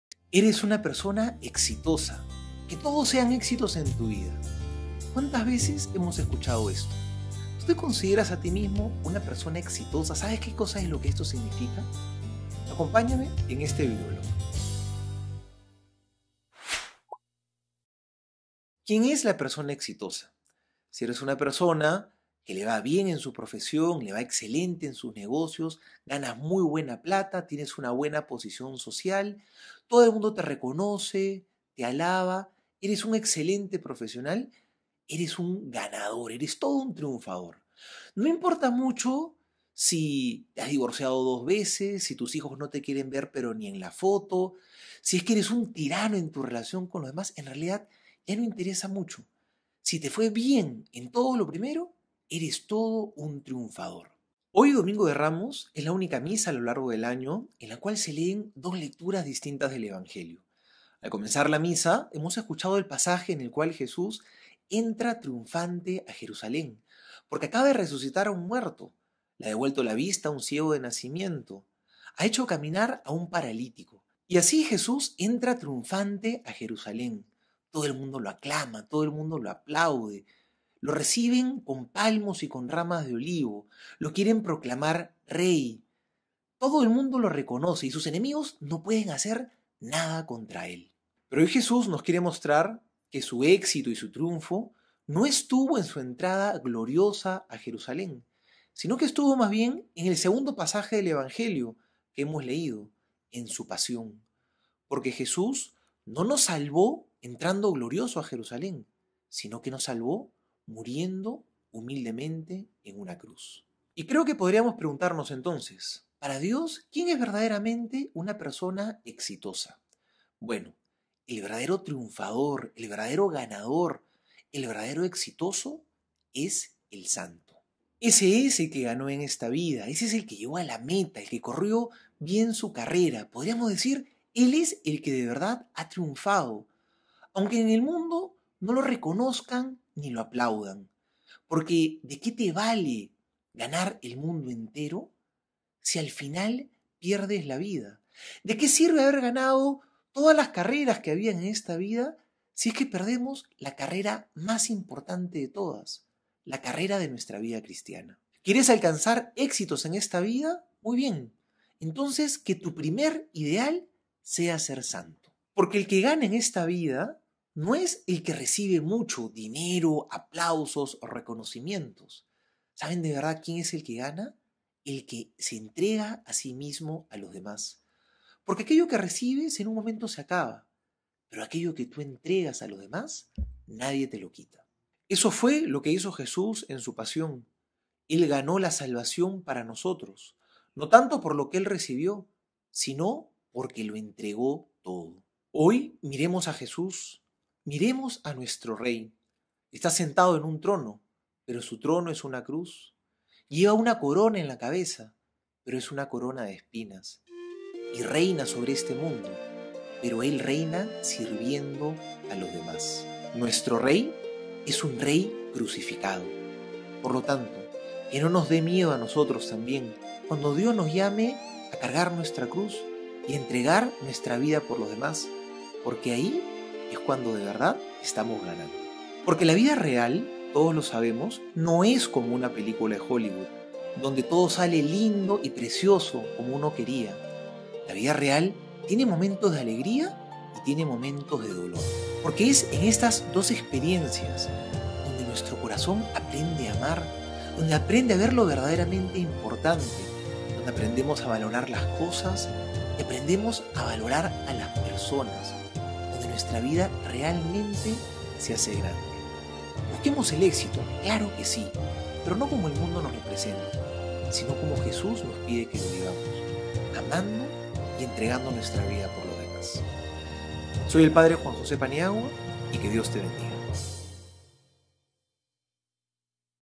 Jesús, ¿cómo hago para ser una persona exitosa? - Homilía del domingo de Ramos
Puedes seguir también los comentarios al Evangelio del día desde el fanpage de Facebook o en audio desde los podcasts diarios en spotify:

Texto del Evangelio  según San Mateo 21, 1-11

Cuando se acercaban a Jerusalén y llegaron a Betfagé, junto al monte de los Olivos, Jesús mandó dos discípulos, diciéndoles:
—Id a la aldea de enfrente, encontraréis en seguida una borrica atada con su pollino, desatadlos y traédmelos. Si alguien os dice algo contestadle que el Señor los necesita y los devolverá pronto.
Esto ocurrió para que se cumpliese lo que dijo el profeta:
«Decid a la hija de Sión: Mira a tu rey, que viene a ti, humilde, montado en un asno, en un pollino, hijo de acémila.»
Fueron los discípulos e hicieron lo que les había mandado Jesús: trajeron la borrica y el pollino, echaron encima sus mantos y Jesús se montó. La multitud extendió sus mantos por el camino;algunos cortaban ramas de árboles y alfombraban la calzada.
Y la gente que iba delante y detrás gritaba:
—¡Viva el Hijo de David!
—¡Bendito el que viene en nombre del Señor!
—¡Viva el Altísimo!
Al entrar en Jerusalén, toda la ciudad preguntaba alborotada:
—¿Quién es éste?
La gente que venía con él decía:
—Es Jesús, el profeta de Nazaret de Galilea.